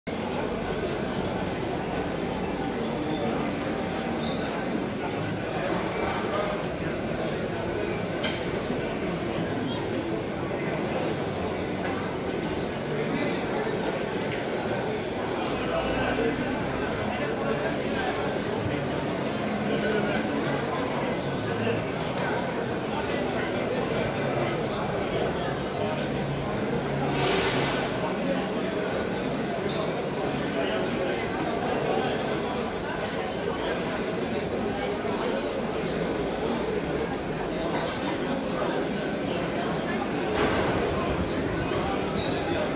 Çayyolu Antika Pazarı
Unique turntables and tape recorders.